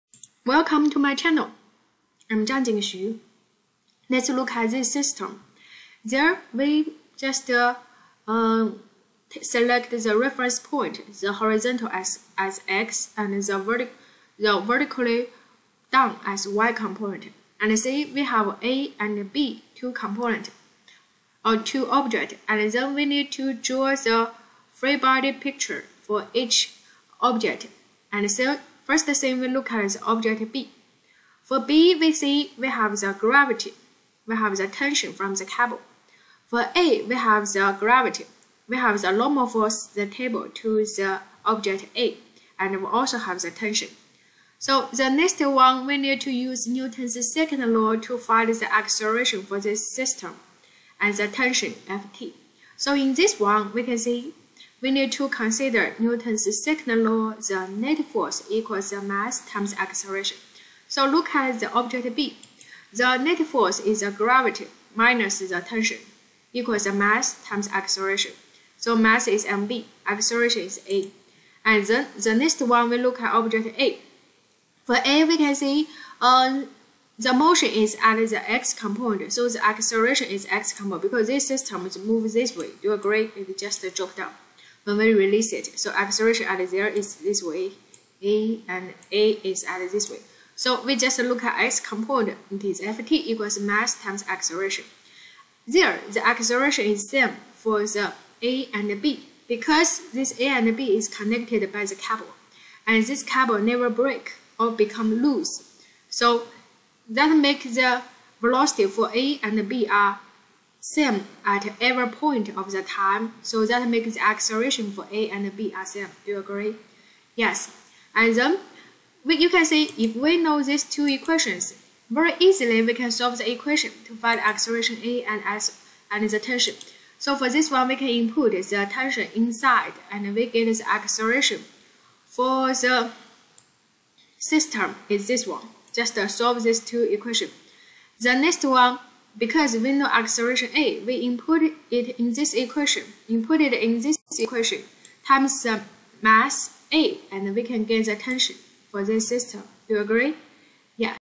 (4-51) Figure 4-45 shows a block (mass mA) on a smooth horizontal surface, connected by a thin cord
(4-51) Figure 4-45 shows a block (mass mA) on a smooth horizontal surface, connected by a thin cord that passes over a pulley to a second block (mass mB), which hangs vertically, (a) Draw a free-body diagram for each block, showing the force of gravity on each, the force (tension) exerted by the cord, and any normal force. (b) Apply Newton’s second law to find formulas for the acceleration of the system and for the tension in the cord. Ignore friction and the masses of the pulley and cord.